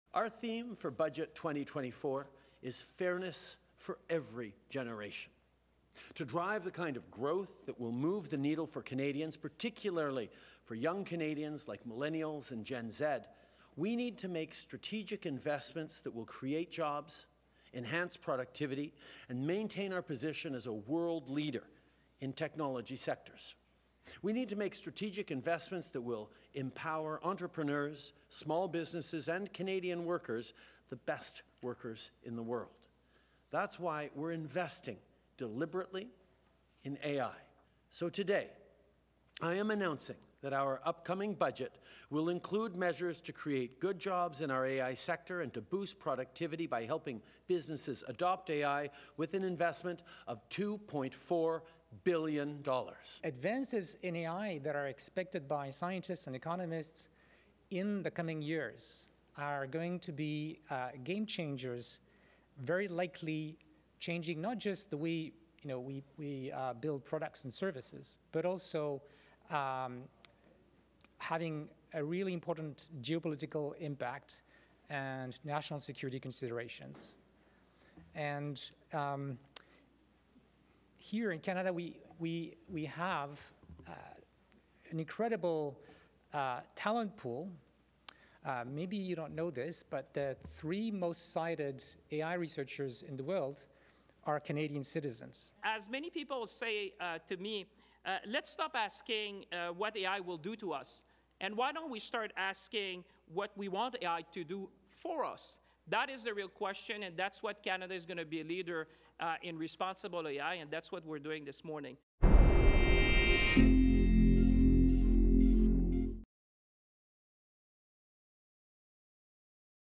Prime minister announces 2.4 billion to build up Canada's AI capacity
Prime Minister Justin Trudeau has announced $2.4 billion to boost Canada's artificial intelligence capacity. Trudeau made the announcement as part of his government's pre-budget tour and says the majority of the money is going into a fund that will aim to provide access to computing capabilities and technical infrastructure. (April 7, 2024)